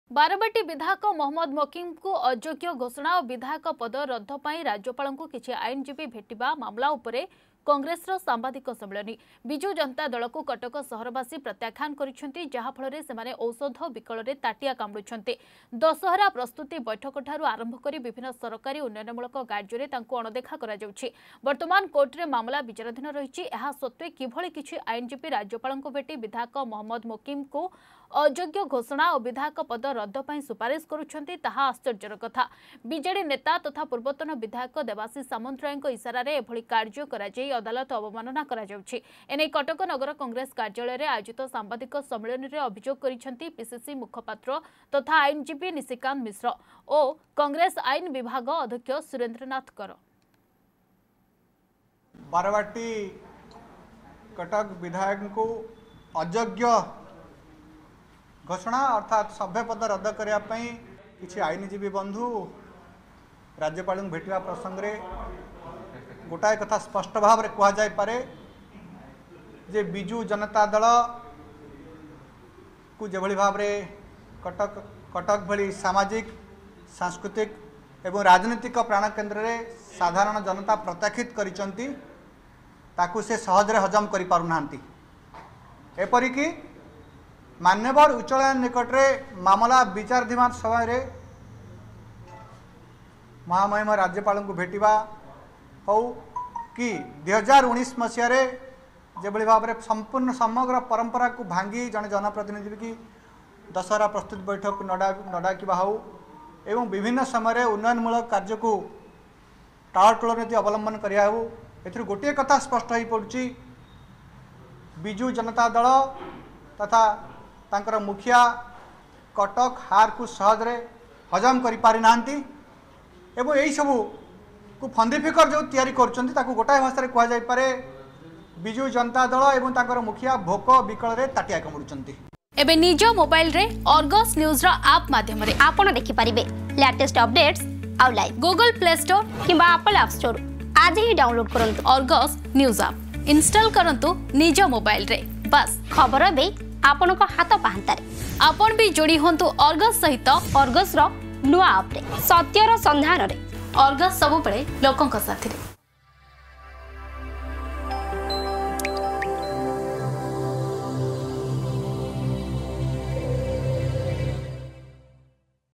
କଂଗ୍ରେସର ସାମ୍ବାଦିକ ସମ୍ମିଳନୀ...
ବାରବାଟୀ ବିଧାୟକ ମହମ୍ମଦ ମୋକିମଙ୍କୁ ଅଯୋଗ୍ୟ ଘୋଷଣା ଓ ବିଧାୟକ ପଦ ରଦ୍ଧ ପାଇଁ ରାଜ୍ୟପାଳଙ୍କୁ କିଛି ଆଇନଜୀବୀ ଭେଟିବା ମାମଲା ଉପରେ କଂଗ୍ରେସର ସାମ୍ବାଦିକ ସମ୍ମିଳନୀ। ବିଜୁ ଜନତା ଦଳକୁ କଟକ ସହରବାସୀ ପ୍ରତ୍ୟାଖାନ କରିଛନ୍ତି। ଯାହାଫଳରେ ସେମାନେ ଔଷଧ ବିକଳରେ ତାଟିଆ କାମୁଡ଼ୁଛନ୍ତି...